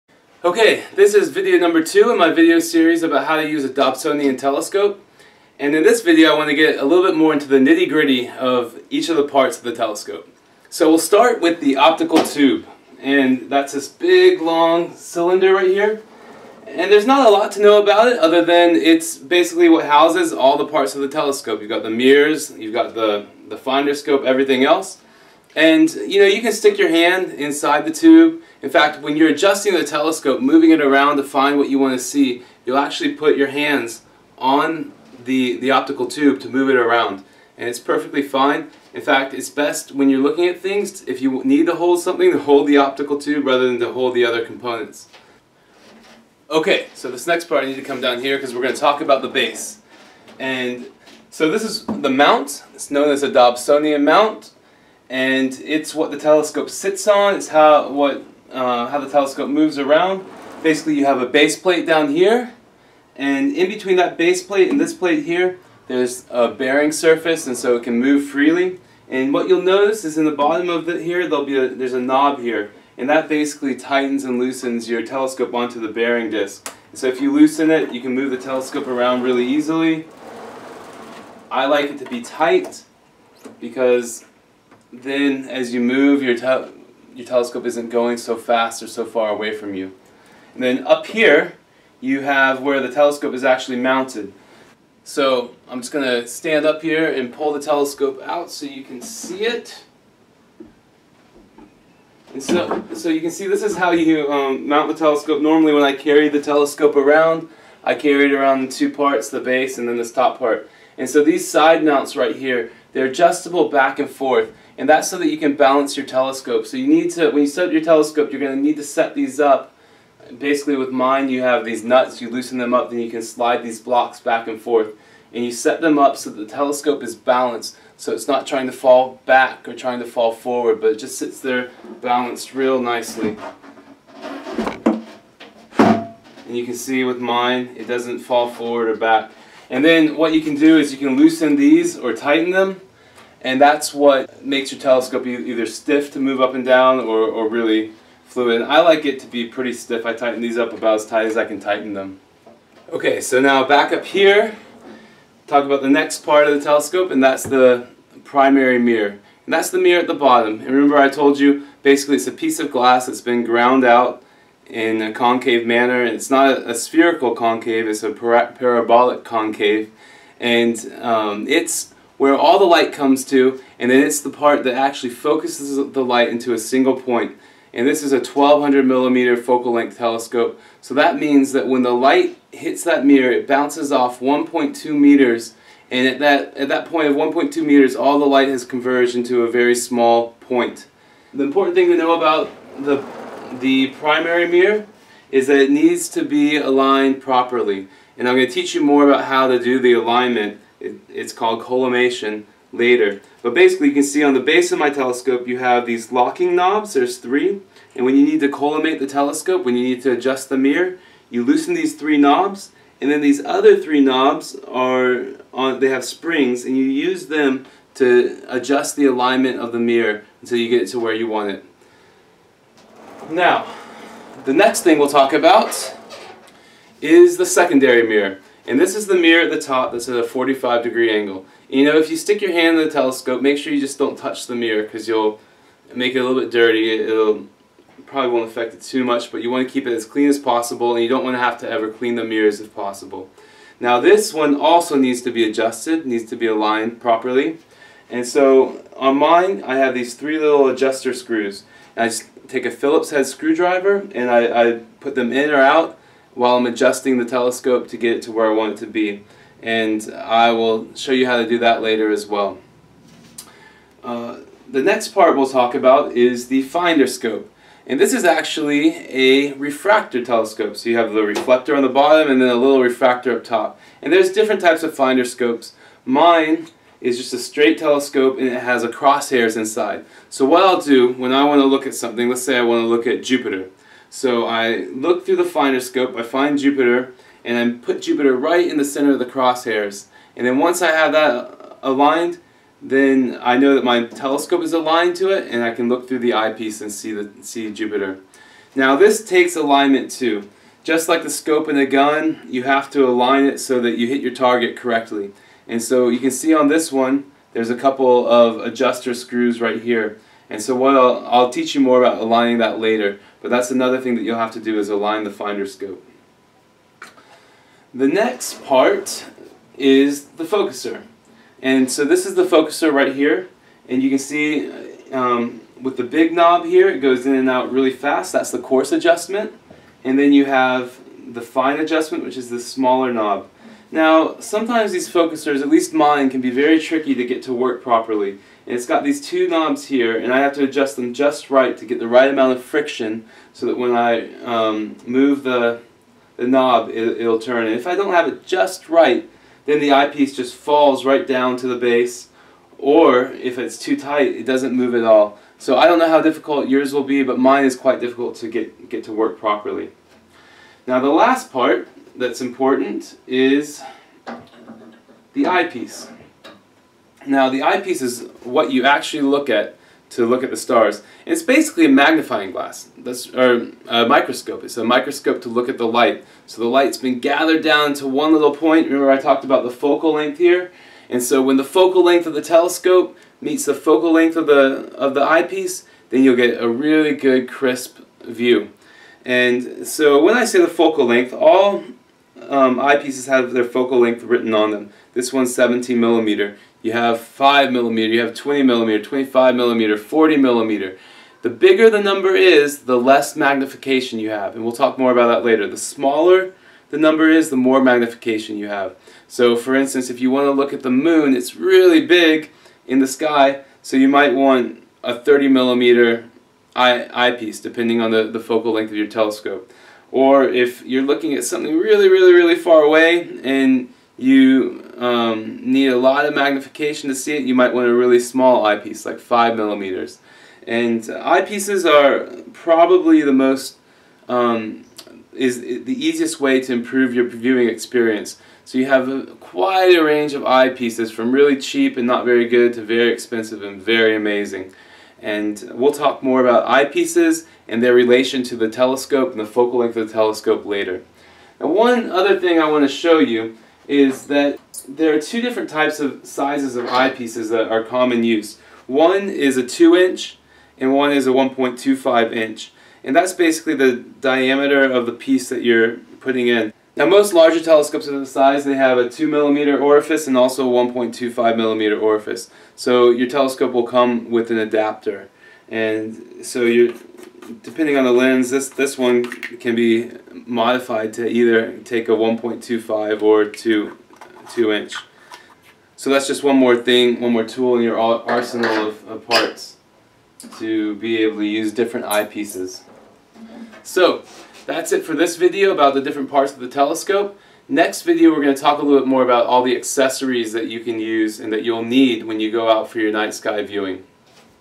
Amateur Astronomy Video 2- Parts of the Telescope
This is the second video in a series I am making for my nieces and nephews.  It is all about the parts of a Dobsonian Mount Reflector Telescope. To see the next video in the series all about telescope accessories follow this